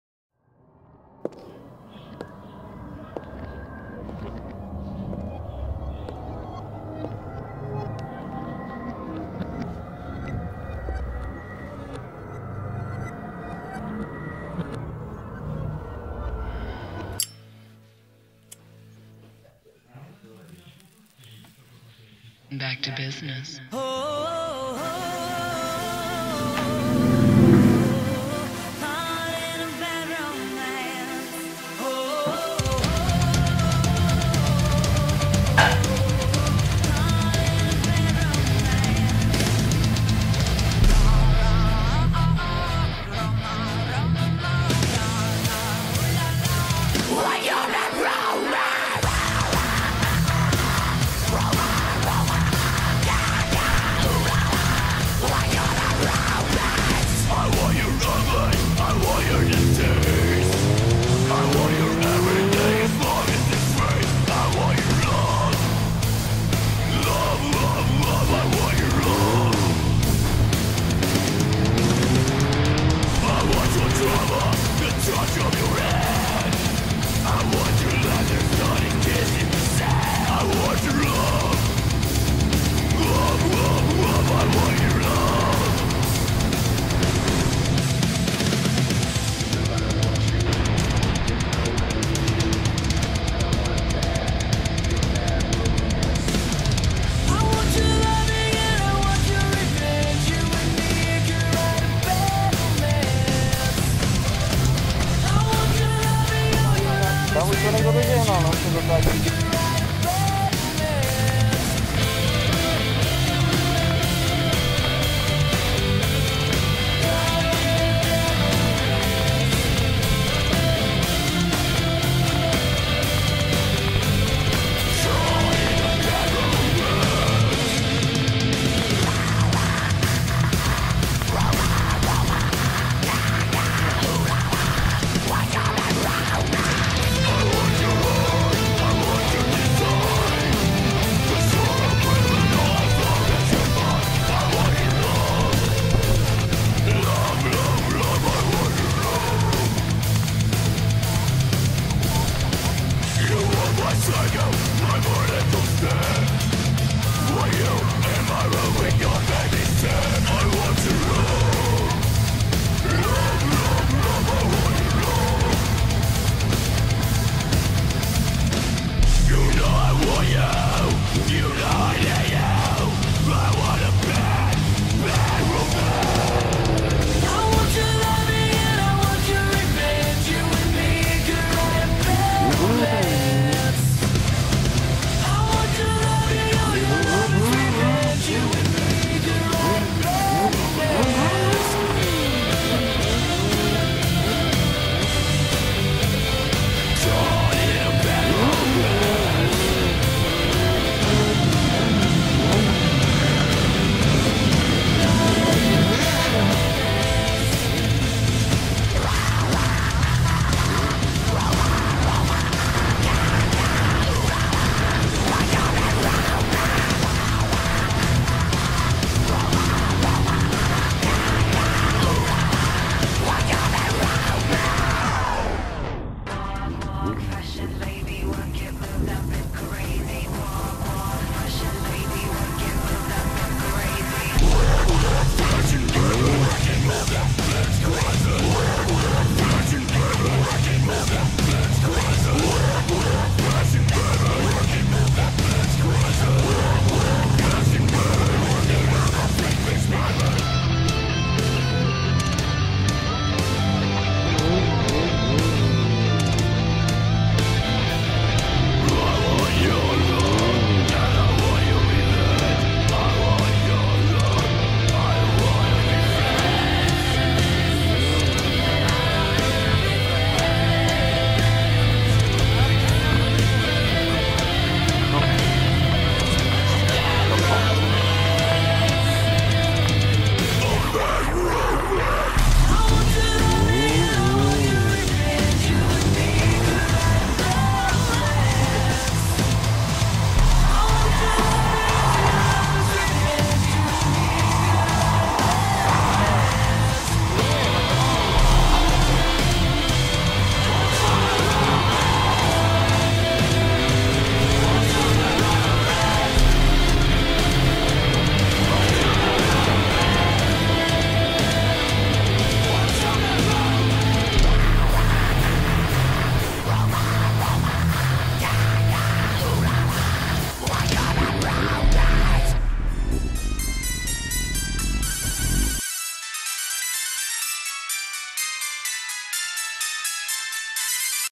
Žehnání motorek Vacenovice 2014
Menší sestřih motovýletu na Žehnání motorek ve Vacenovicích